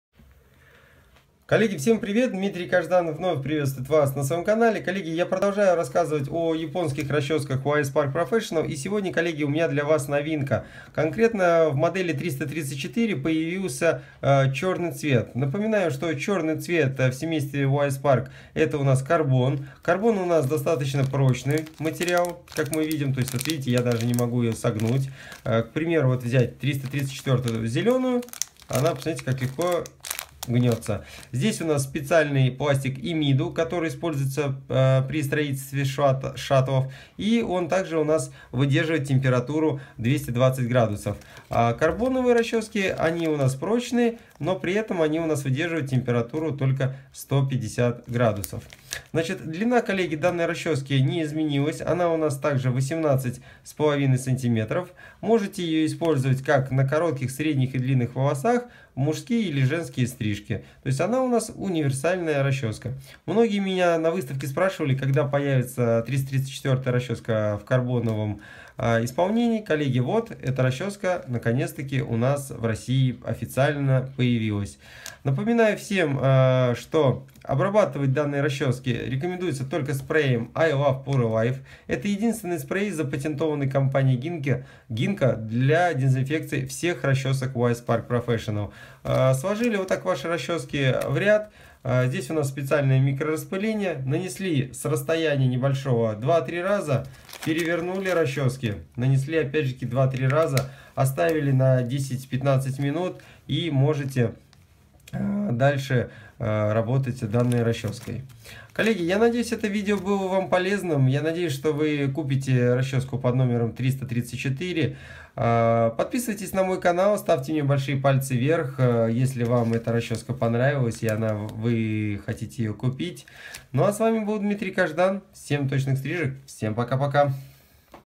Расческа Y.S. Park 334 - карбон
Японская расческа  Y.S. Park 334 - это парикмахерский инструмент.
Y.S. Park professional 334, производится из специального пластика " ИМИДУ" . Который выдерживает температура 220 градусов . Так Расческа Y.S. Park 334 есть и в "Карбоне" который выдерживает температуру 150 градусов. При этом  карбон достаточно прочный материал.
Расческа Y.S. Park 334 подходит для мужских стрижек , женских стрижек.
Длин Расчески Y.S. Park 334 составляет 18.5 см
Цвета расчески Y.S. Park 334 ,  зеленая , белая , розовая
Y.S. Park 334 официальный сайт ShopCombs.Ru